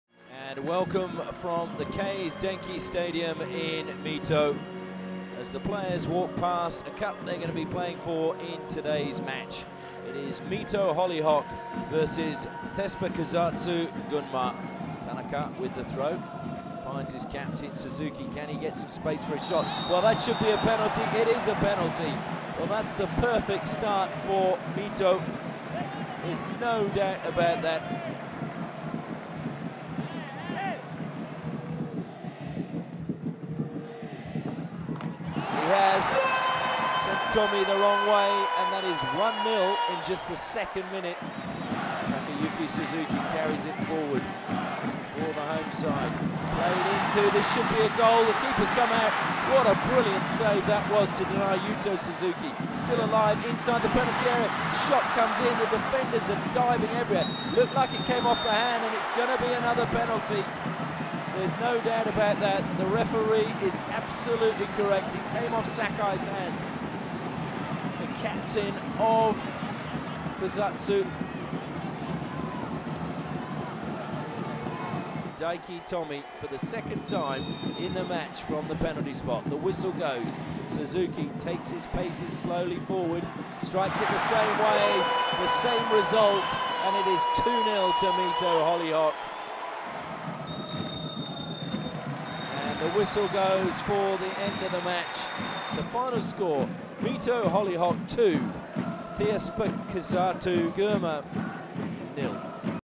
Mito Hollyhock vs Thespakusatsu Gunma: J League Division 1 (Round 22)
Mito Hollyhock 2-0 Thespakusatsu Gunma, goals and highlights
Y. Suzuki (PG) 3'
Y. Suzuki (PG) 63'